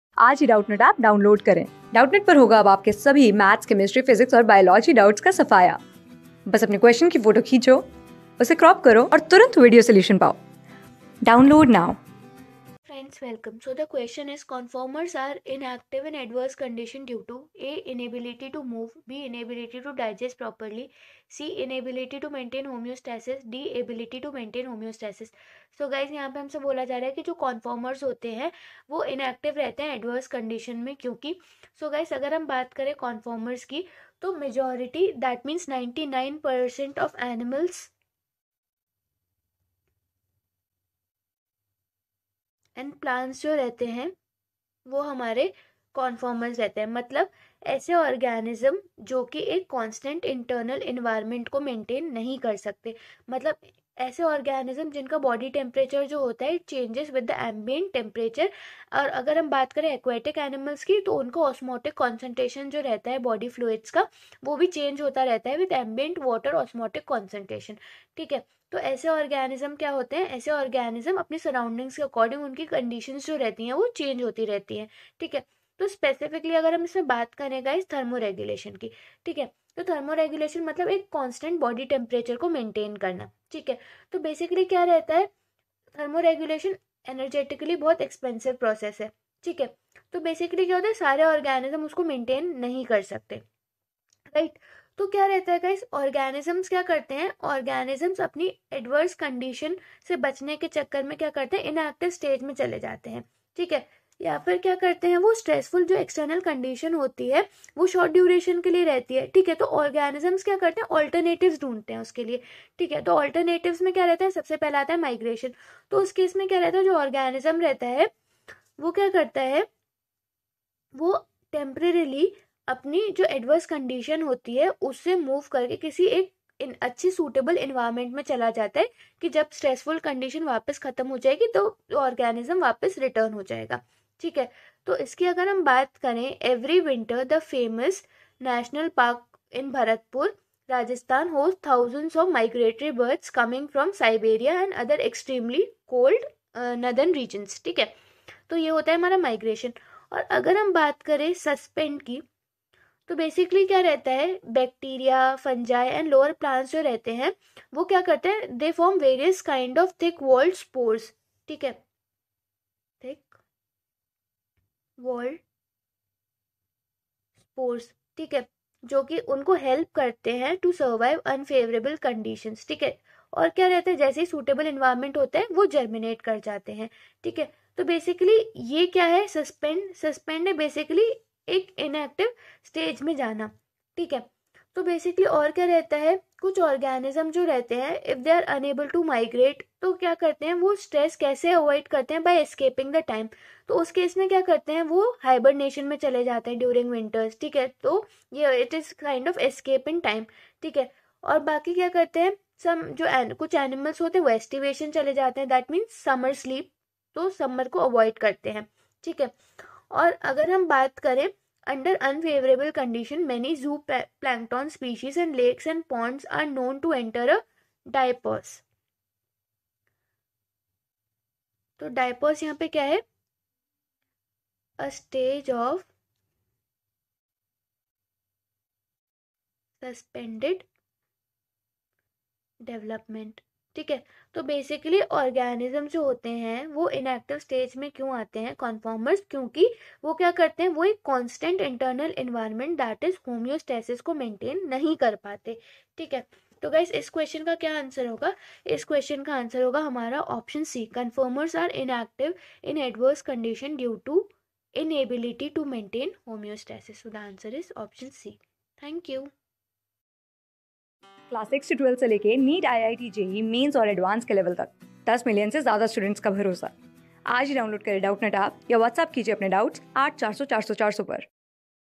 Conformers are inactive in adverse conditions due to | CLASS 11 | ORGANISMS AND POPULATIONS | B...
Conformers are inactive in adverse conditions due to
Class: 11
Subject: BIOLOGY
Chapter: ORGANISMS AND POPULATIONS
Board:IIT JEE

👉 Have Any Query? Ask Us.